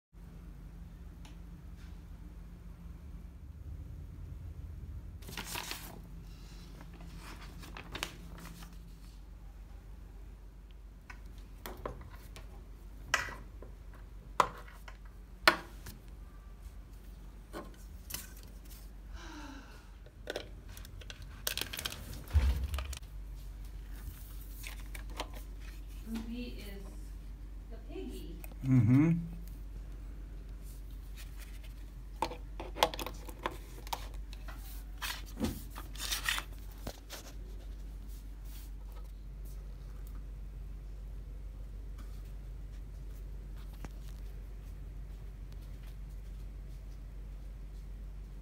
LiteFuze Brand Step Up and Step Down Transformers for Singapore-NEED for 110V Plug In Appliances
Stuck or need help? Call me right now and show me your problem
This is very important you pick at least a couple of these up prior to your arrival unless you want to pay more than you normally would for one if you can even find one. The power outlets are very different in Singapore than in the U.S. so you're appliances, lamps, TV's etc. will NOT work unless you plug it into one of these. We were blindsided by this upon initial arrival and so much so that we were not able to charge our phones right away and you definitely need a phone in Singapore. You will need one of these in Singapore if you plan on running your 110V lamps, drills, toasters, coffee makers, hair dryers, etc. One place to buy them is from amazon.

Here's the link (paid amazon link):